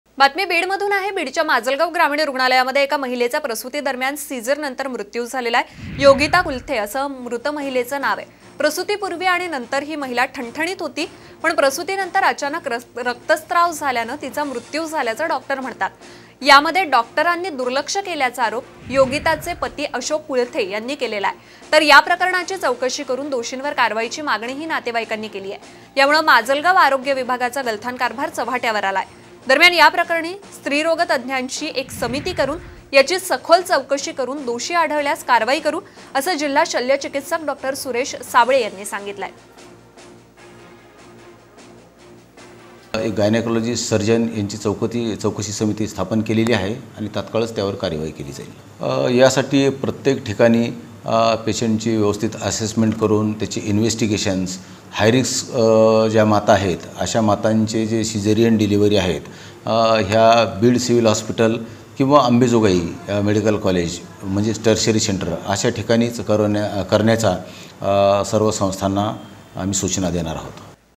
Majalgaon Shocking News : रुग्णालयात महिलेचा प्रसूतीदरम्यान अचानक रस्क्तस्राव वाढल्यानं मृत्यू
Watch Latest News Updates on Majalgaon Shocking News : रुग्णालयात महिलेचा प्रसूतीदरम्यान अचानक रस्क्तस्राव वाढल्यानं मृत्यू